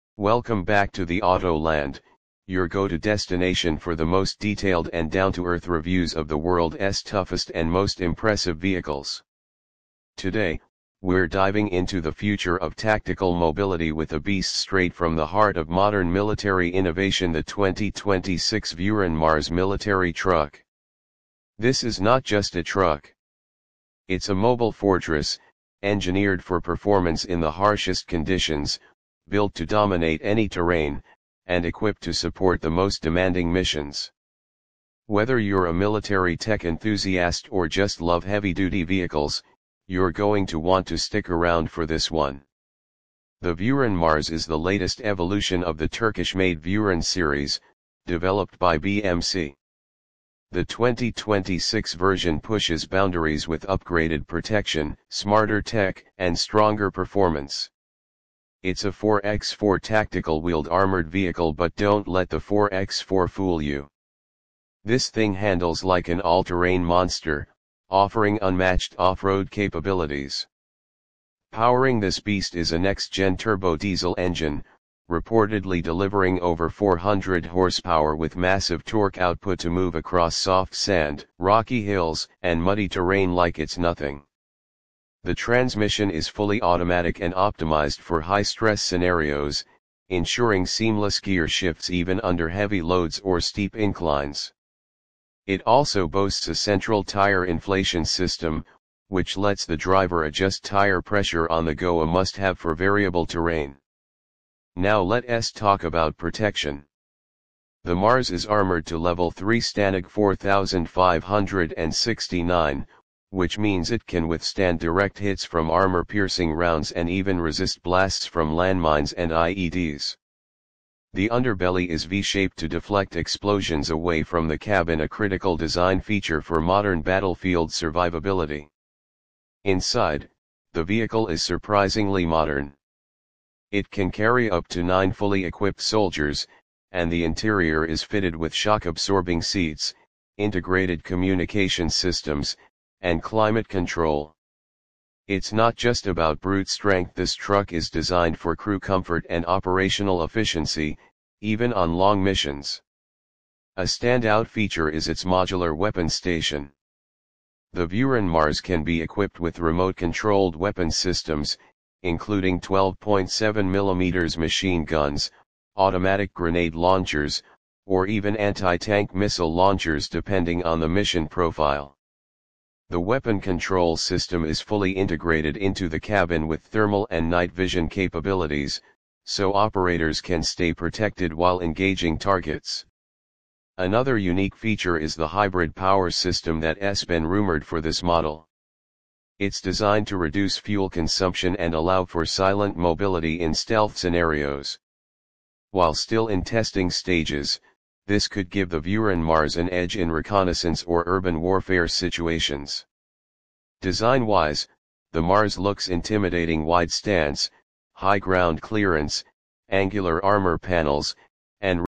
2026 TREVA30 Military Truck – The Most Advanced Combat Vehicle Yet?
In this video, we take an in-depth look at the 2026 TREVA30, a next-gen military truck that’s redefining what battlefield logistics and mobility look like. With a 720-horsepower turbo-diesel engine, hybrid capabilities, armored monocoque cab, and modular adaptability, the TREVA30 isn't just a transport vehicle—it's a frontline force multiplier.

We cover everything from performance specs, off-road capabilities, advanced tech features, and its unique modular design. Whether you're into defense technology, military vehicles, or rugged engineering, this beast is one to watch.